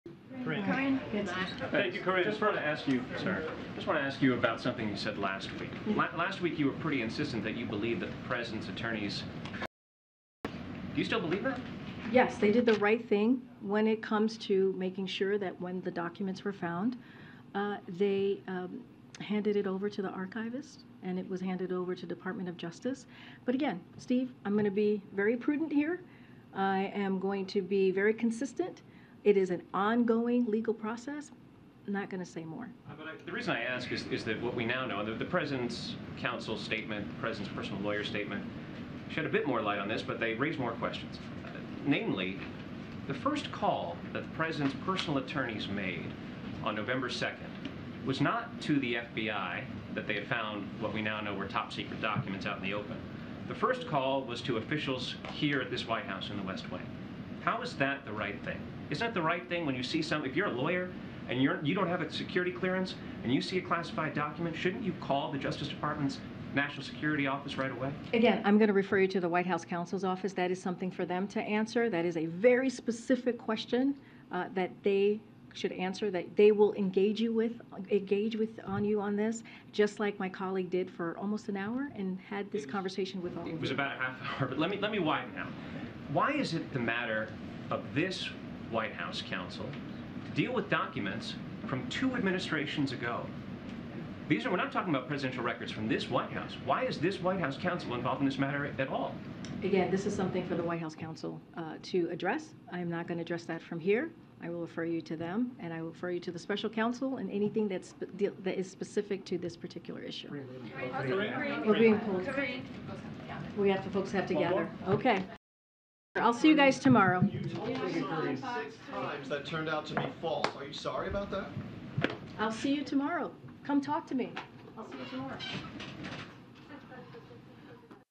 Do you believe the Presidents Attorneys who's first call was to call WH instead of the DOJ, did the
Do you believe the Presidents Attorneys who's first call was to call WH instead of the DOJ, did the right thing. White House Press Conference, Jan 17, 2023
White House press secretary Karine Jean-Pierre, Continues deflect and refuse to answer questions on the Classified Documents that were found.